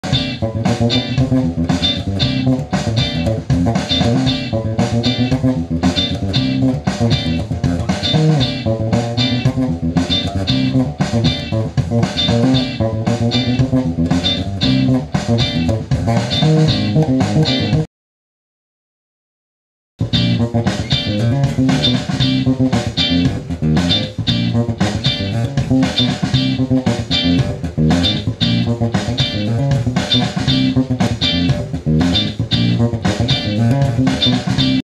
Two Fusion Basslines (Separated by an abrupt crossfade)
Just practicing a different style, kind of a Jaco-esque staccato fingerstyle funk.

Two different sixteenth note Funk/Fusion basslines in the style of Jaco Pastorius/Francis Rocco Prestia.  Excuse the terrible crossfade between the two lines, I could not undo it because iMovie had limited edit undo functions in memory.

It is not a perfect performance, but I don't care that much.

Playing a Fiesta red Fender Road Worn Jazz Bass, stock with GHS Boomers.